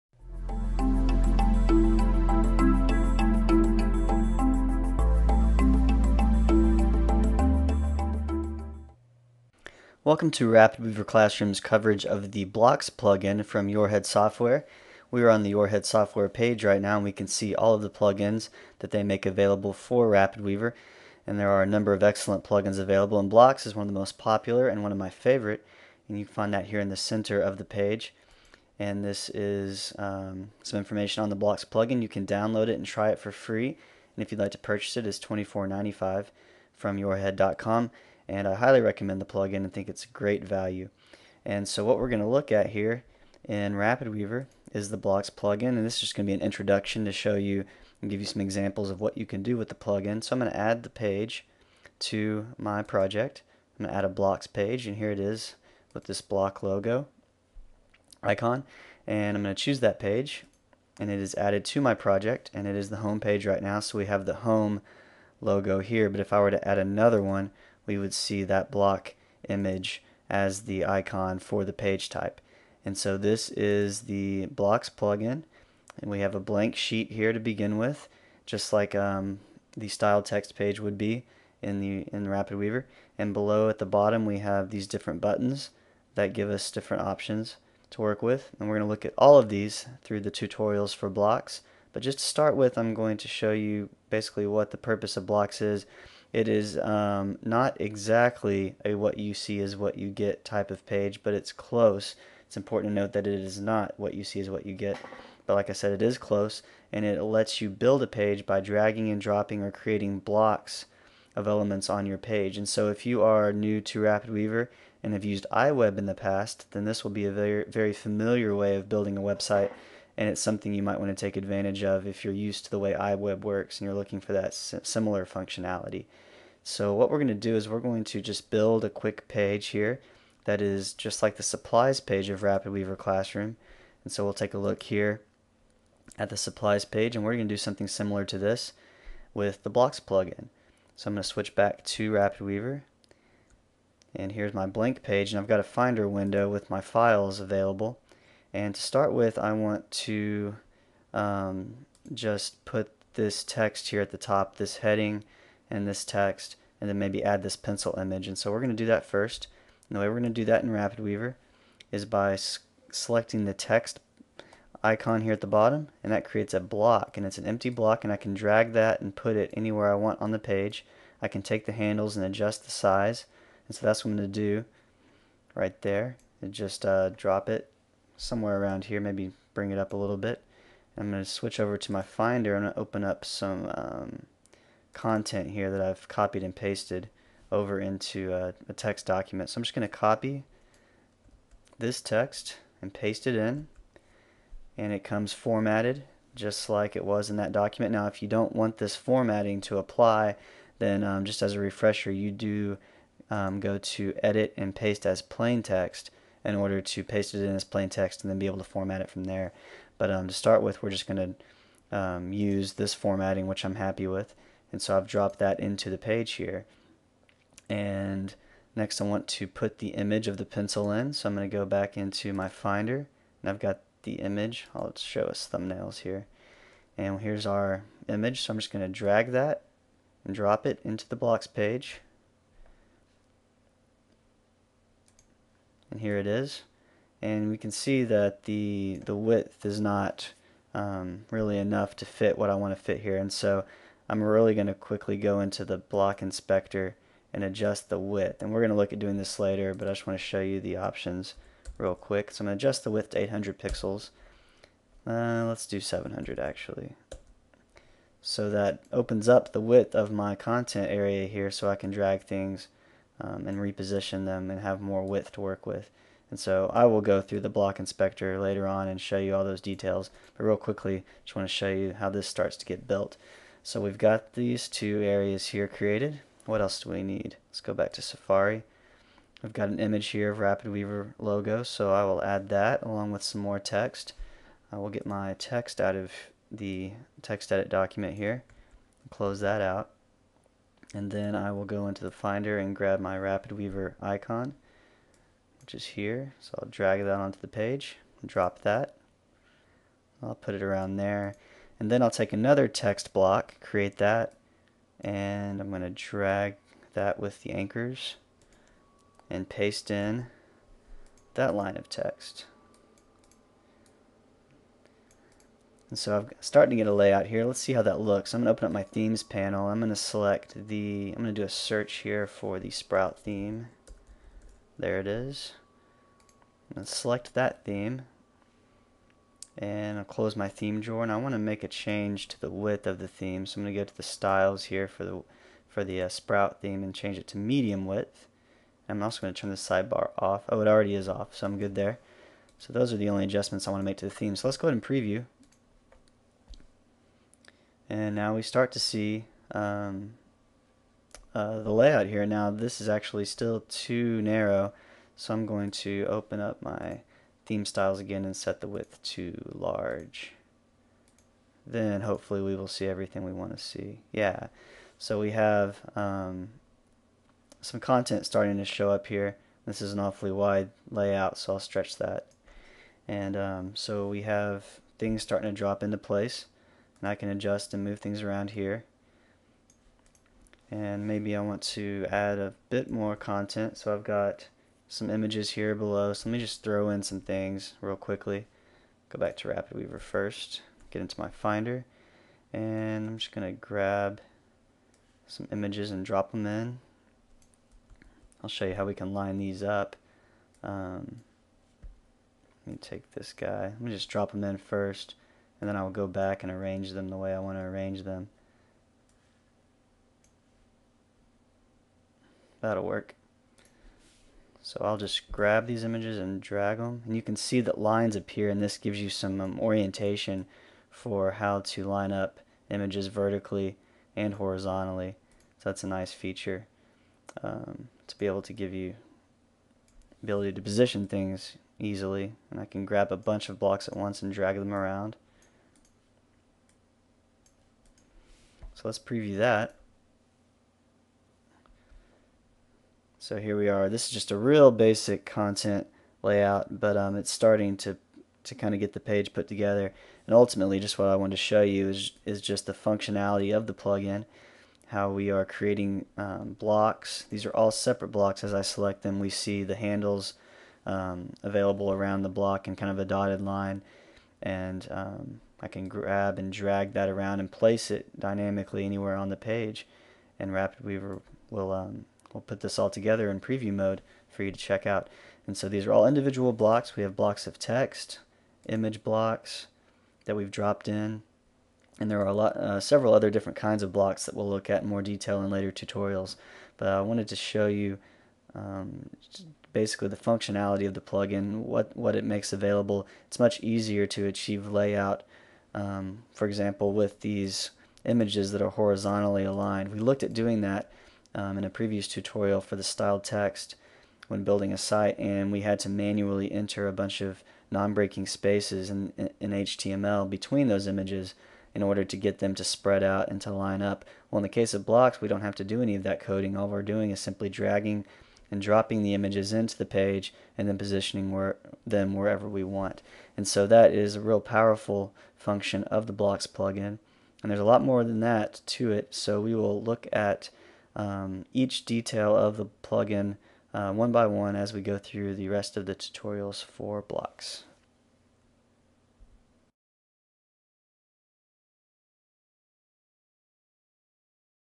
RapidWeaver HD Screencast: Introduction to the Blocks Plugin
RapidWeaver Classroom presents a tutorial introducing the Blocks plugin for RapidWeaver from YourHead Software.